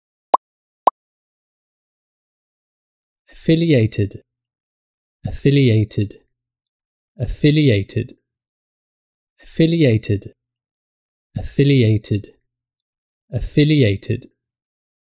How to Pronounce Affiliated
In this video we'll show you how to correctly pronounce:  Affiliated.